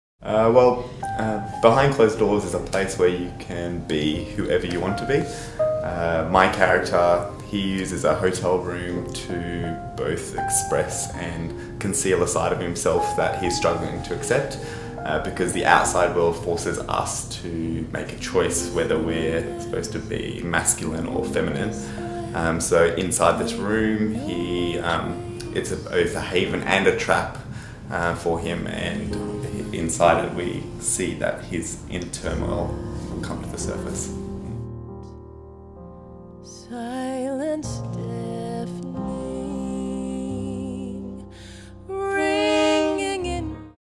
Natalie Weir's Behind Closed Doors - The Business Man (Richard Causer)
Natalie Weir's Behind Closed Doors 19 - 27 May 2017, Playhouse QPAC

Presented by Expressions Dance Company and Queensland Performing Arts Centre.

In this stunning work from choreographer Natalie Weir, Behind Closed Doors draws you into a hotel where the private lives of staff and guests are exposed.

Showcasing breathtakingly beautiful and athletic dance by the exceptional Expressions Dance Company, this poetic kiss-and-tell reveals the darkness, playfulness and fragility of human nature.

Behind Closed Doors features seductive live jazz performed by Queensland’s Trichotomy, with special guests, vocalist Kristin Berardi and saxophonist Rafael Karlen.